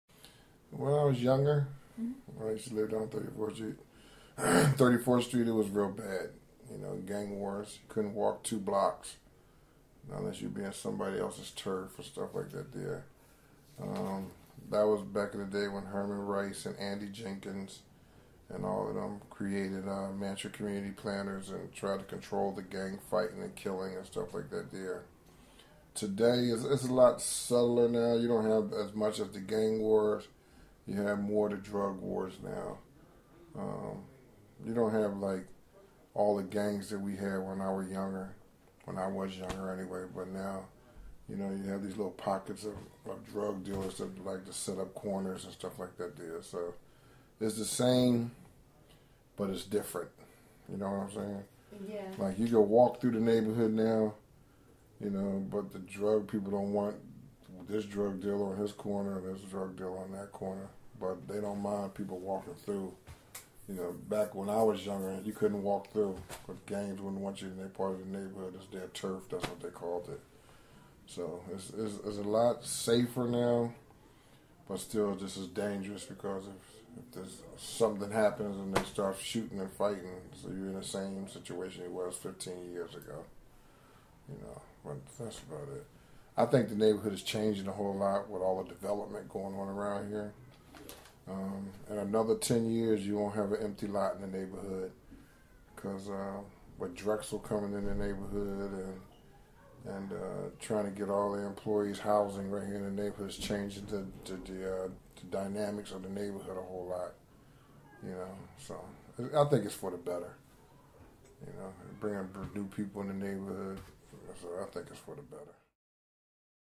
Richard Purdie
A native of Mantua, Richard Purdie is the president of the Mantua Haverford Community Center, located on 39th Street between Mt. Vernon and Wallace Streets. Purdie just recently retired from the Electric Company on February 25, 2014, where he worked for 35 years. He has also been the president of the community center for 15 years, where he oversees the day-to-day activities, chases the kids around, and chairs the board meetings and programs located at the center. As a prominent leader in the neighborhood of Mantua, Purdie is concerned about Mantuaís young people, and is in the process of opening a Catholic School at the Mantua Haverford Community Center.

Richard Purdie views the change that Drexel Universityís expansion is bringing with optimism: he thinks that the Drexel has something to offer Mantua, and knows that Mantua has much to offer Drexel. Purdie believes that to have a mutually beneficial relationship, Drexel needs to foster better understanding with the greater neighborhood. This could be done, Purdie suggested, if Drexel explained exactly what they wanted to do in Mantua, listened to what Mantua wants from the University, and then offered Mantua what it wants or needs. Thus far, Purdie sees the changes that Drexel has brought to the neighborhood as positive. Drexel University programs have helped people from Mantua earn their GEDs, absolve their criminal records, and acquire full-time jobs, which has transformed the lives of many Mantua residents. University expansion has filled vacant lots and raised property values in the neighborhood, which Purdie views progress in the neighborhood; and yet, he also recognizes that raised property values adversely affects seniors and other neighbors in Mantua that are on a fixed income.

Like every proud resident of Mantua, Purdie wants what is best for the neighborhood. Purdie firmly believes that the neighborhood must progress, which involves working with Drexel University, or it will be left behind and overpowered by the University.